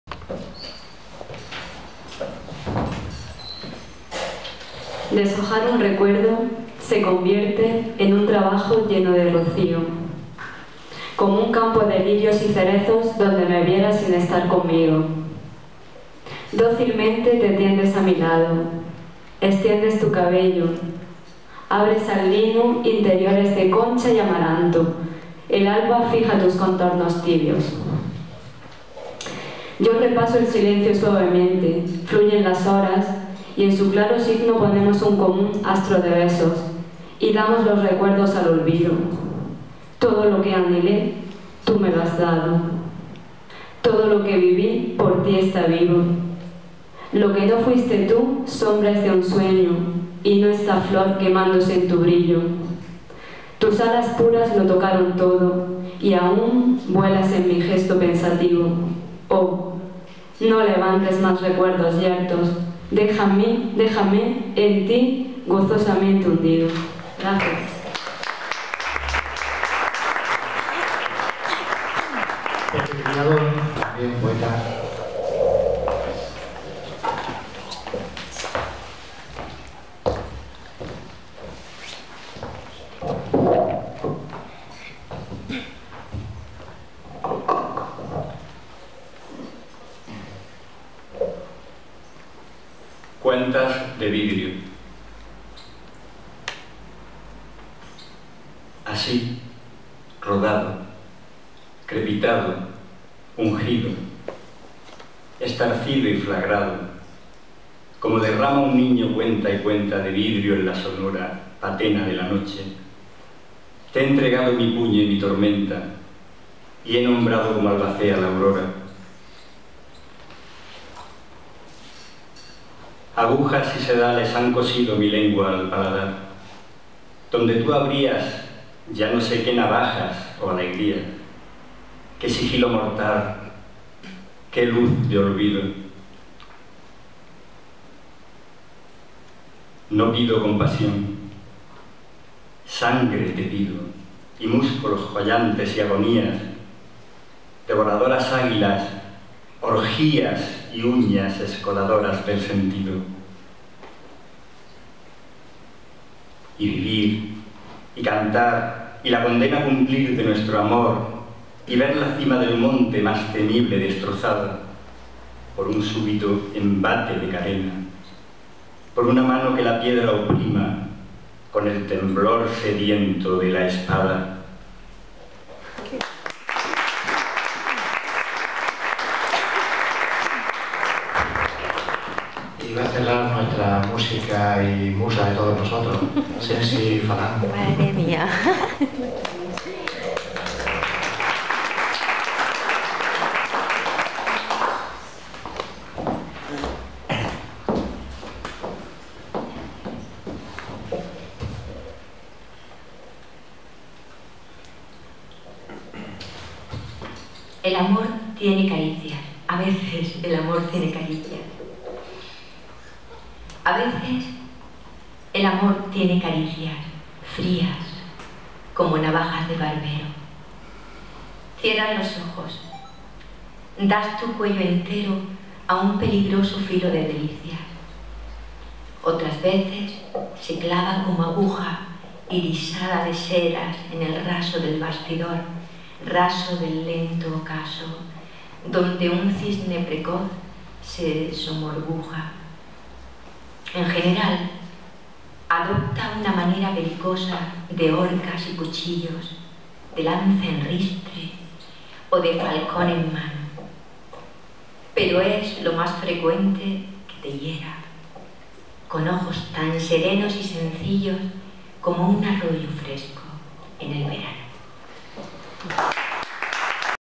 EL POETA ANTONIO CARVAJAL EN ANTAS (8)
Antonio Carvajal presentó su libro "Sol que se alude" en Antas (Almería) invitado por el colectivo Argaria. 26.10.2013.

RECITAL DE POEMAS DE ANTONIO CARVAJAL
.Deshojar un recuerdo se convierte en un trabajo lleno de rocío, leído por Virginia Fernández Collado.
.Cuentas de vidrio, leído por Pepe Criado.
.A veces el amor tiene caricias, leído por Sensi Falán.